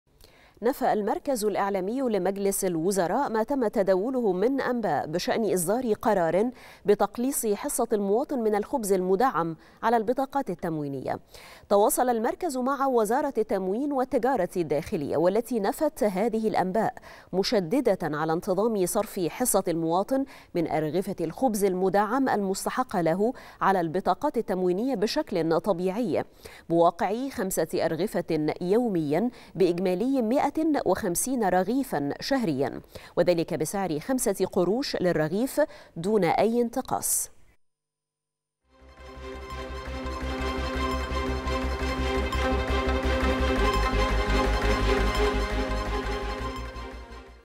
المركز الإعلامي: لا صحة لإصدار قرار بتقليص حصة المواطن من الخبز المدعم على البطاقات التموينية
نشرة الثالثة | المركز الإعلامي لمجلس الوزراء: لا صحة لإصدار قرار بتقليص حصة المواطن من الخبز المدعم على البطاقات التموينية